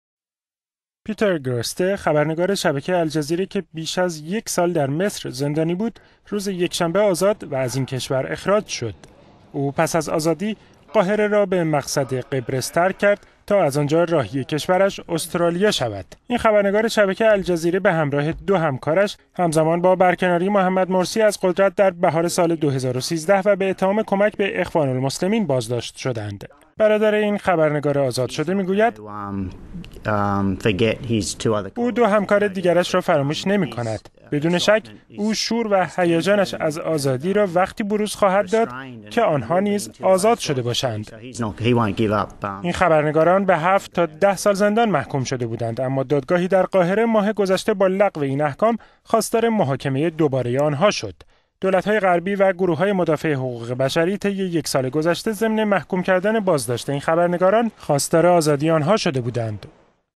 آزادی یکی از سه خبرنگار زندانی شبکه الجزیره در مصر؛ پیتر گرسته در راه استرالیا
پیتر گرسته، خبرنگار شبکه الجزیره که بیش از یکسال در مصر زندانی بود روز یکشنبه آزاد و از این کشور اخراج شد.

او پس از آزادی، قاهره را به مقصد قبرس ترک کرد تا از آنجا راهی استرالیا شود.

این خبرنگار شبکه الجزیره به همراه محمد فهمی و باهر محمد دو همکارش همزمان با برکناری محمد مرسی از قدرت در بهار دو سال گذشته (۲۰۱۳) و به اتهام کمک به اخوان المسلمین بازداشت شدند.

آندروگرسته، برادر خبرنگار آزاد شده گفت:« او دو همکار دیگرش را فراموش نمی کند. بدون شک او شور و هیجانش از آزادی را وقتی بروز خواهد داد که …